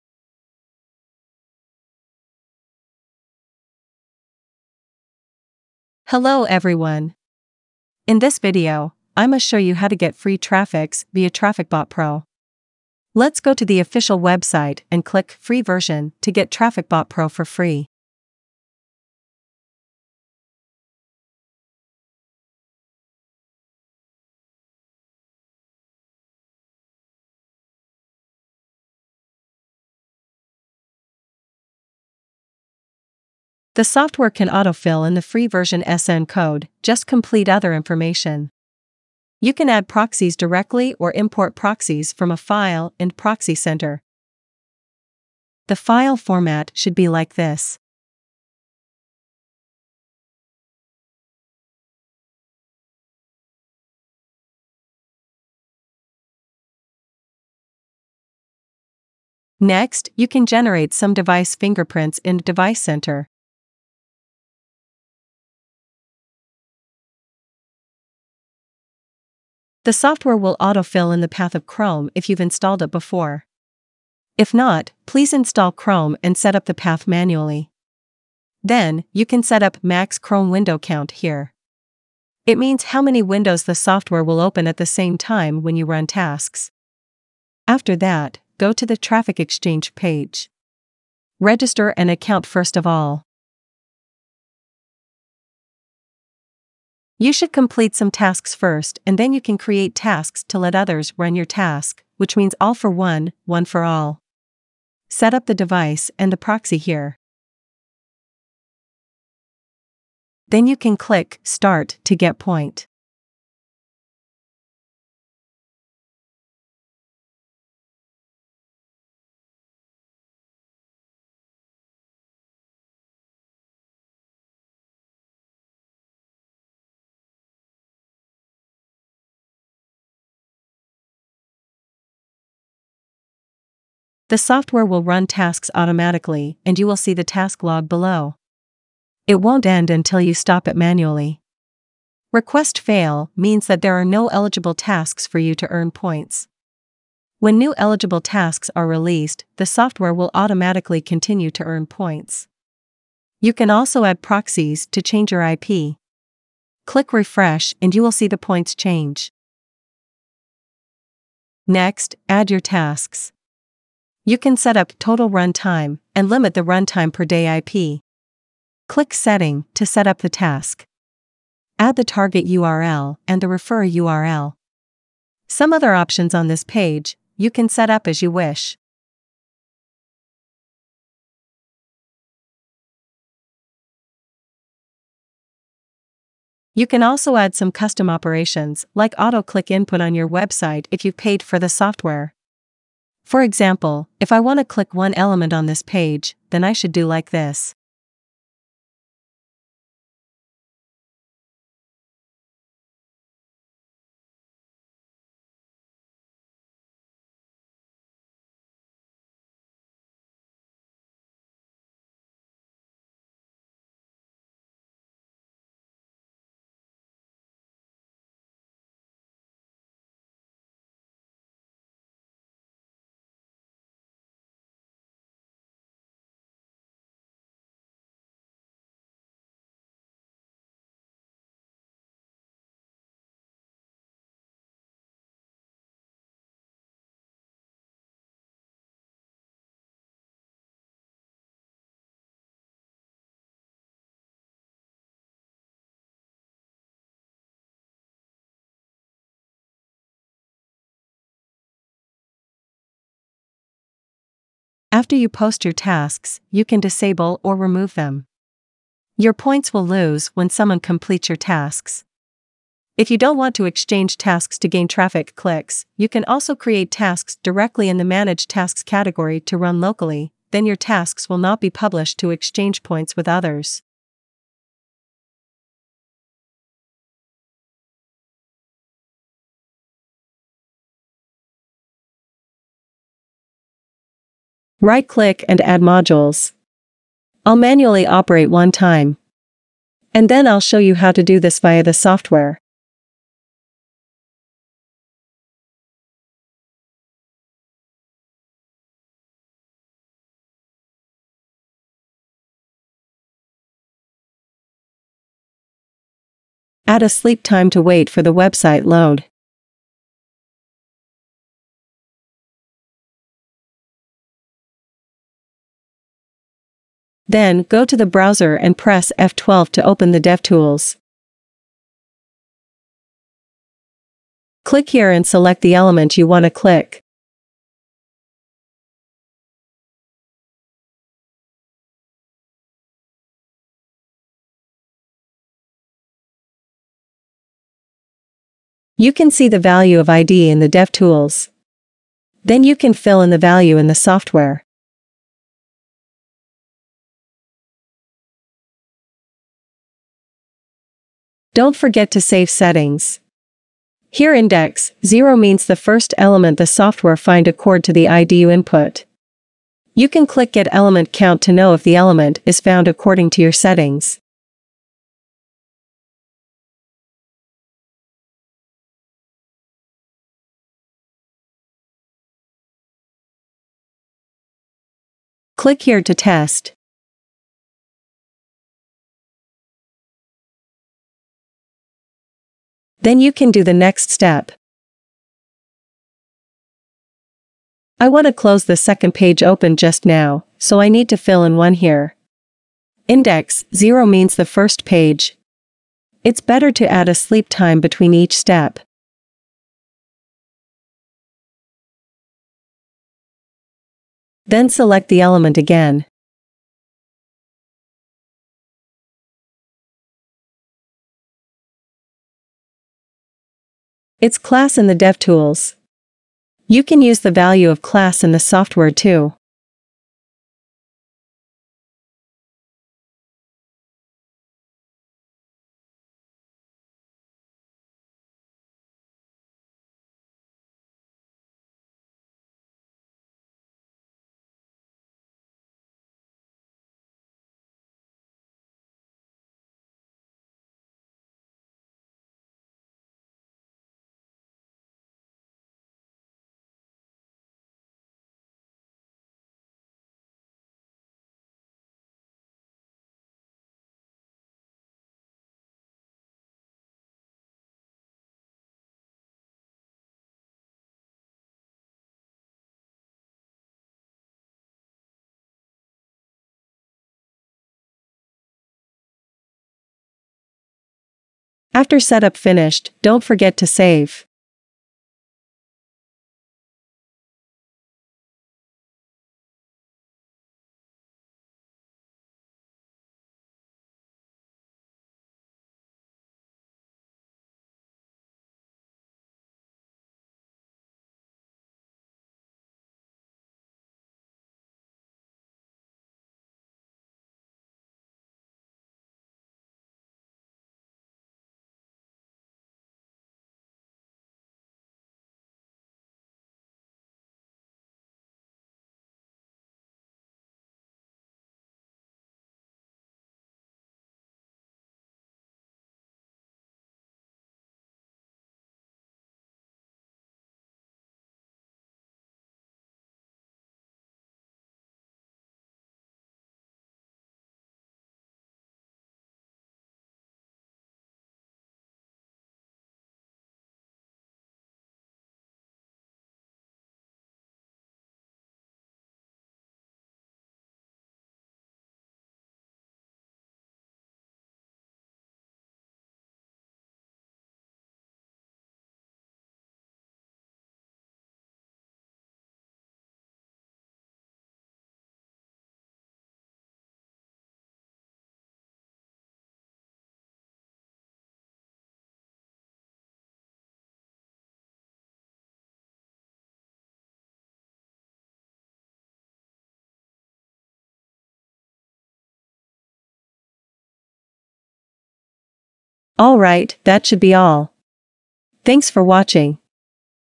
How to get mass real traffic for free? Best Traffic Exchange
00:00 Get the free version
00:39 Config the software
01:51 Get points
02:41 Post the task

Get organic traffic
Automate tasks
Support multi-threading
Anti-Fingerprint
Restrict same IP traffic
Efficient Smart Panel